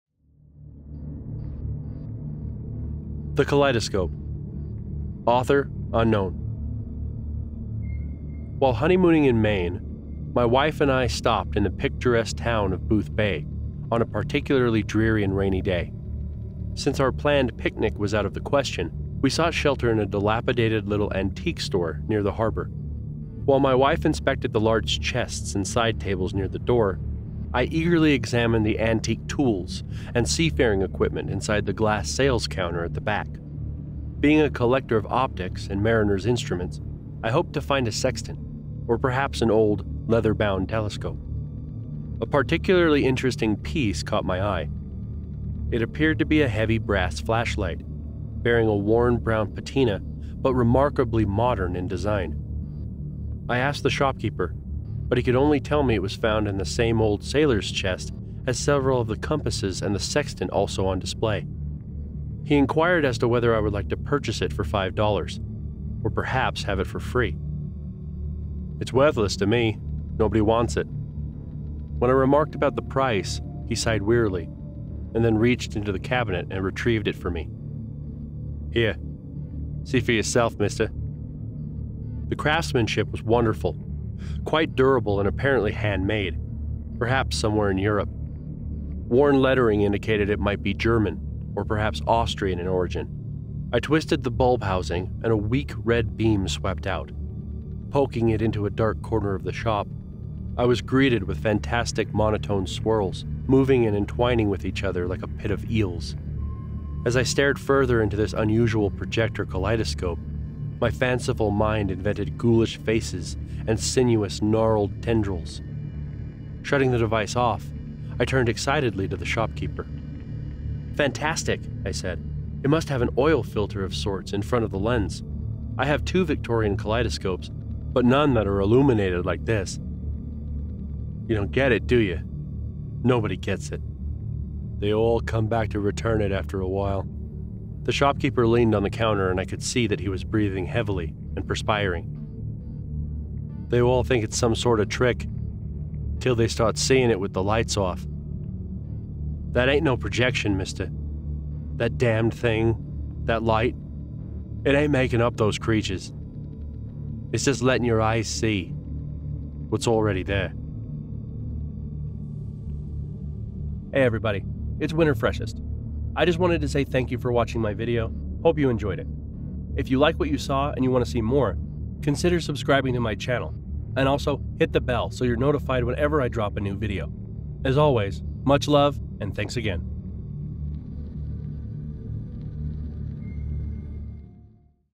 The Kaleidoscope
Narration of the creepypasta The Kaleidoscope. If you enjoy my narrations and have a request, leave a comment on the video with what you'd like to hear.